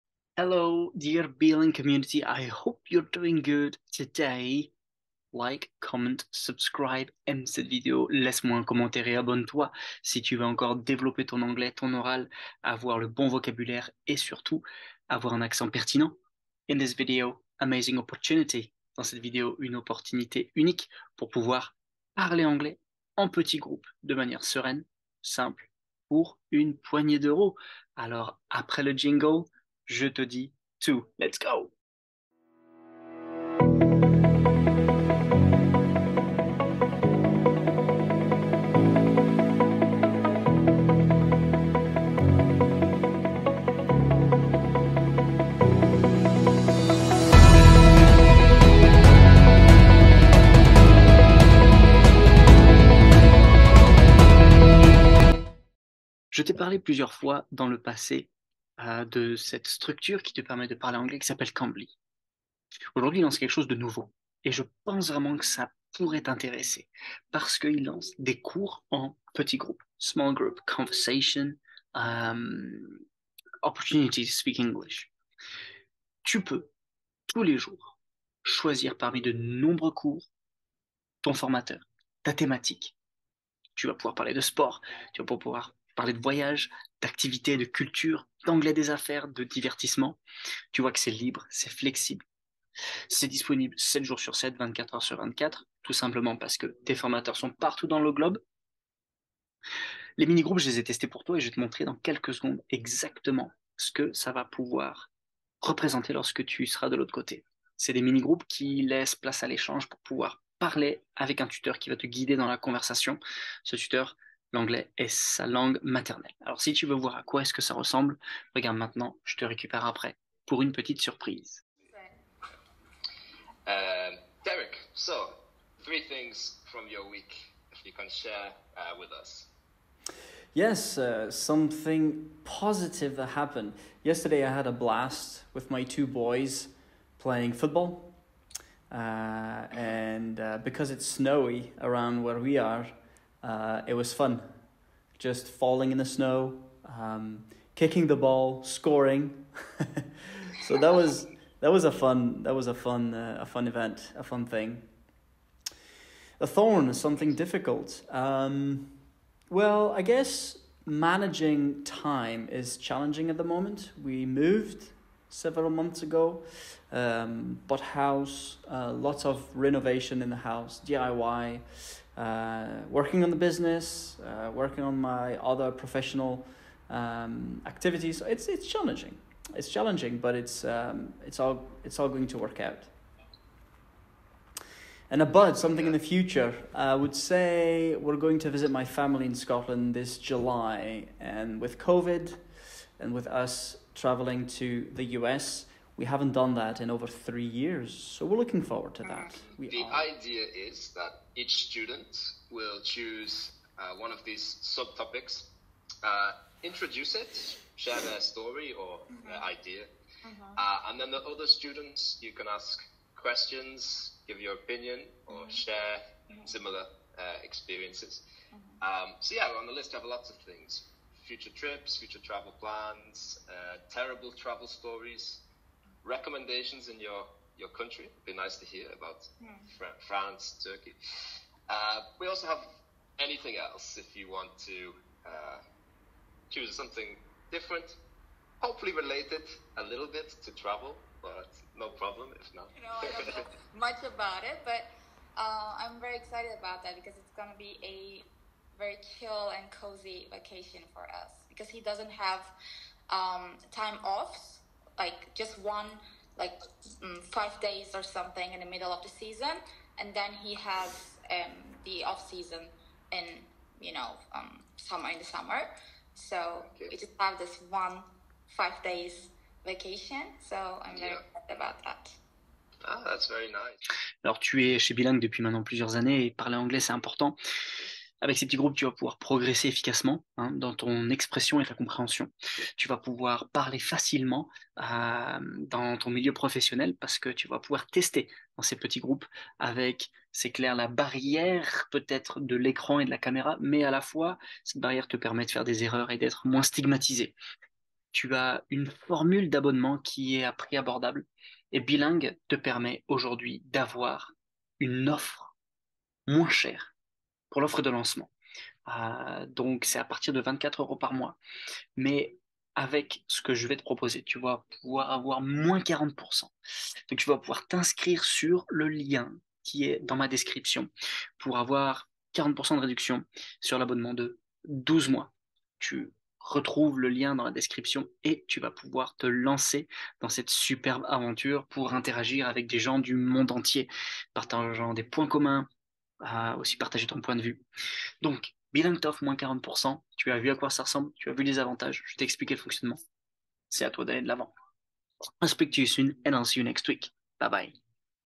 Conversation en petits groupes Cambly! J'ai testé pour toi!
🔊 Par ici pour profiter de l'offre exceptionnelle -40% sur les cours cambly en groupe.